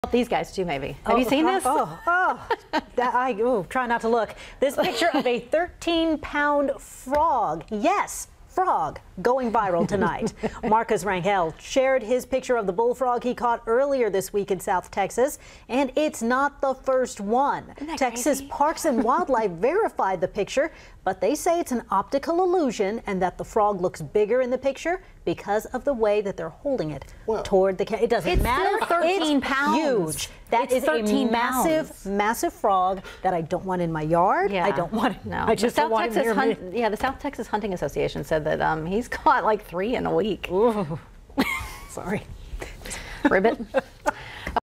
South Texas man catches massive bullfrog
Check this out! This picture of a "13-pound" frog is going viral.